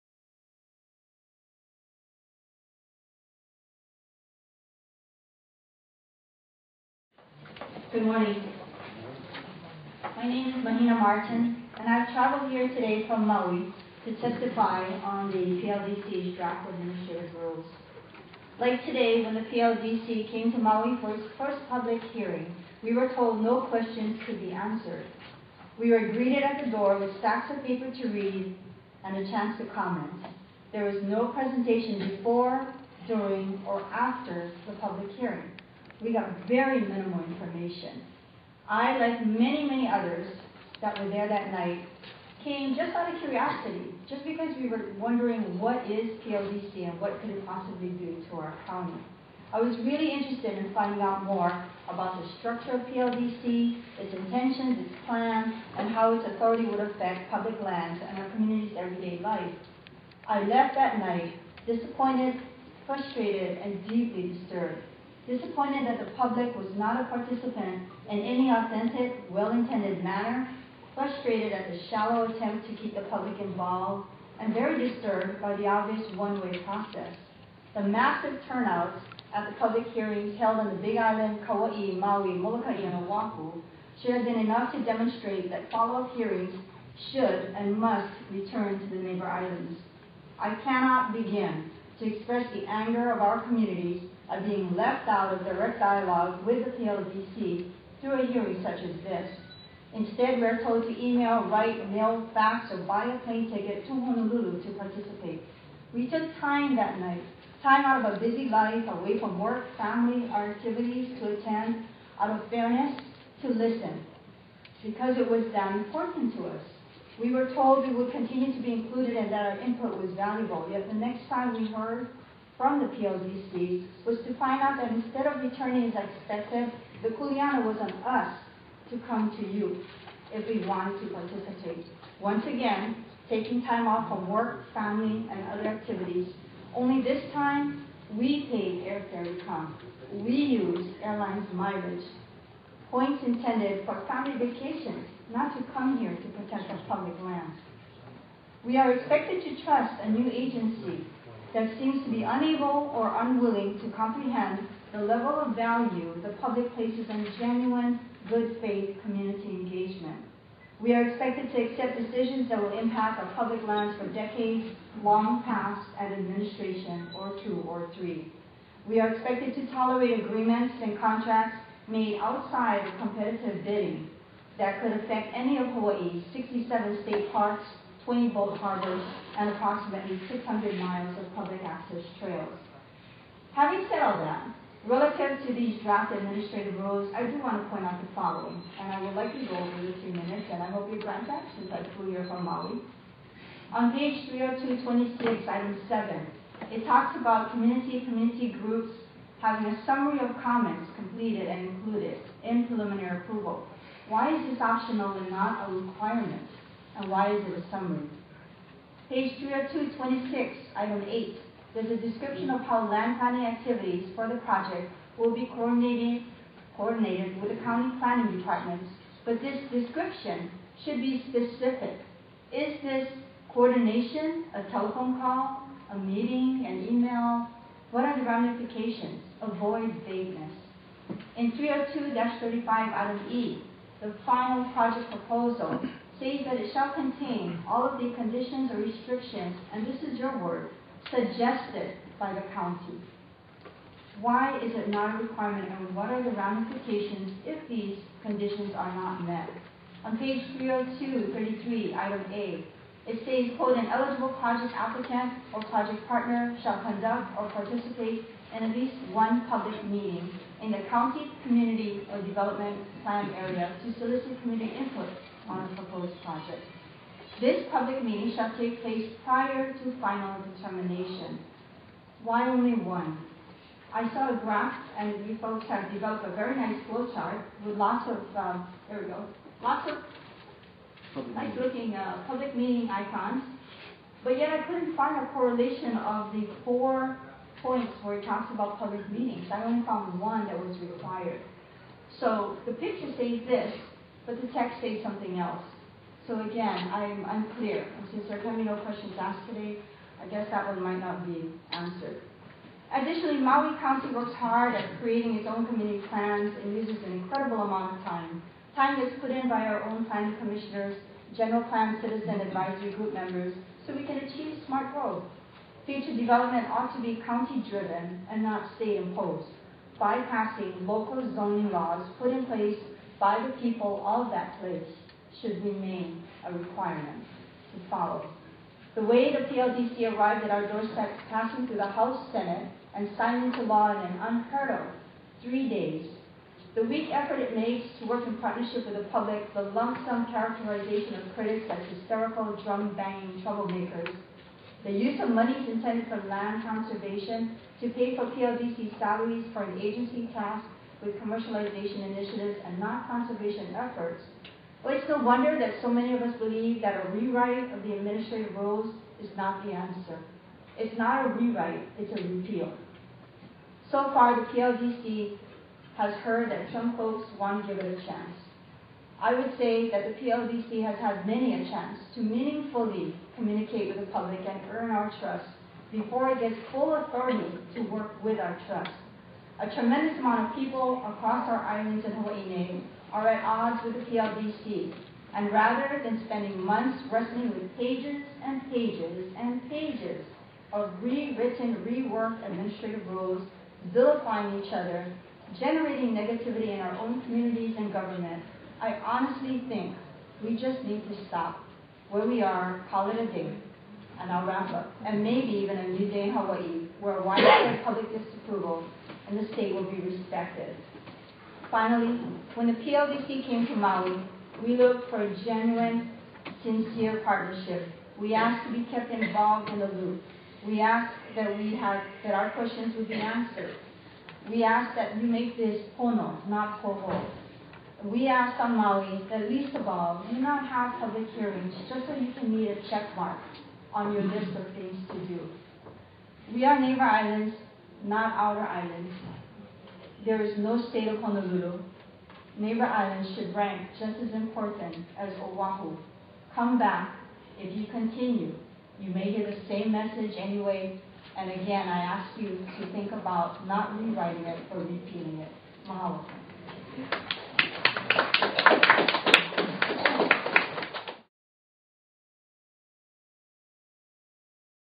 Mahina Martin at PLDC 11-13-2012
From the livestream: Mahina Martin of Maui on the Oahu-centric, Honolulu-centric practice of the PLDC.